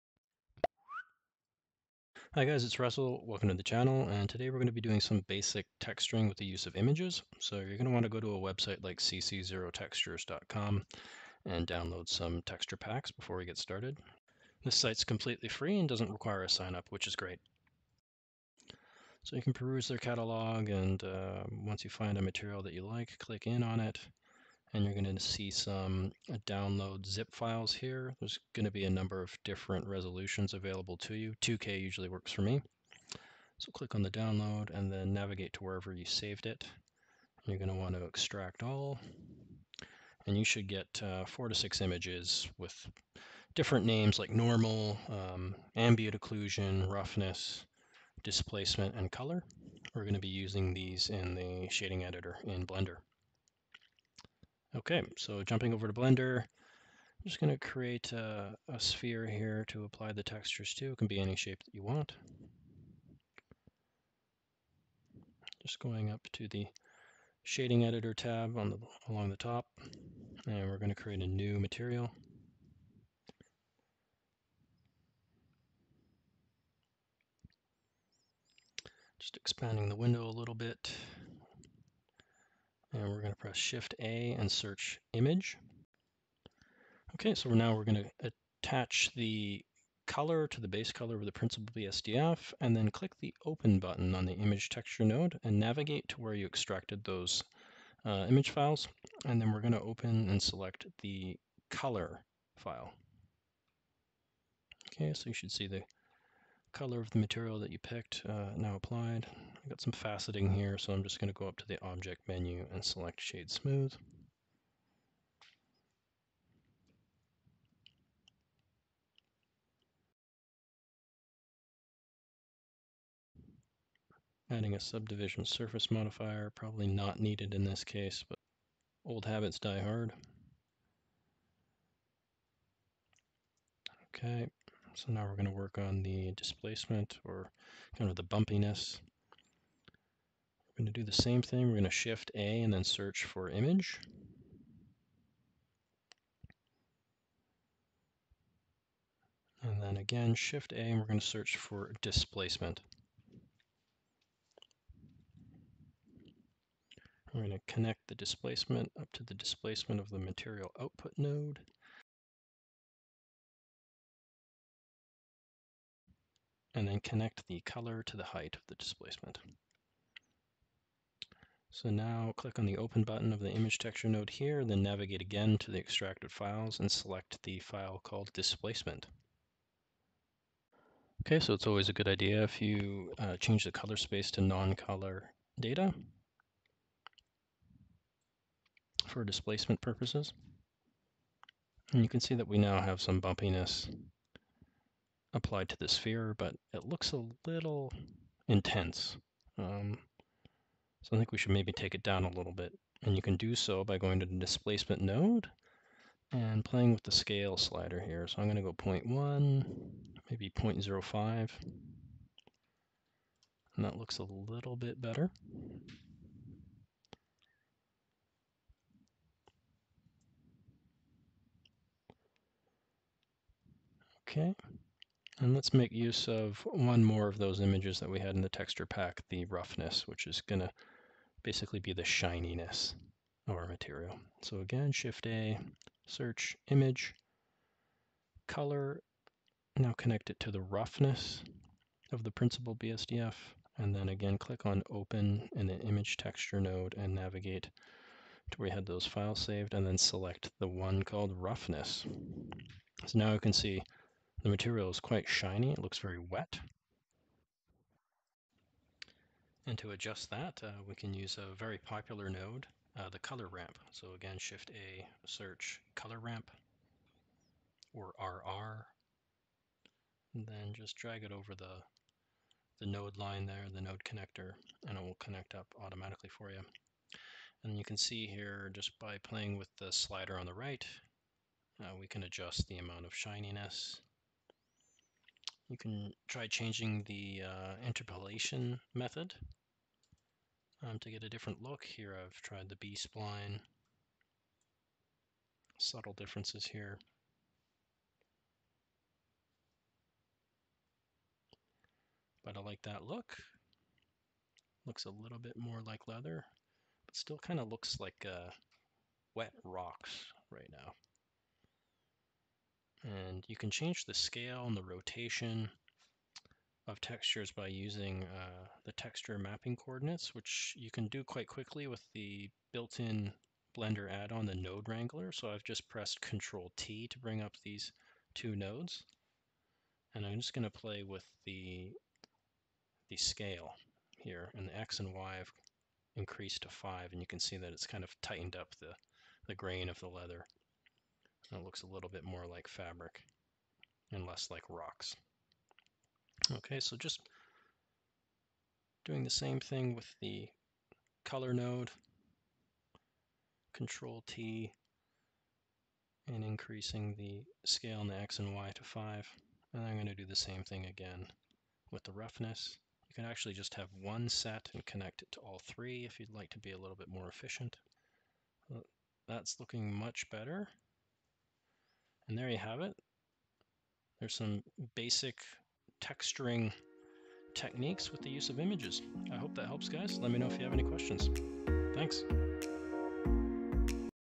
Blender 2.8 Tutorial - Basic Texturing with Images
How to use images for basic texturing in Blender 2.8.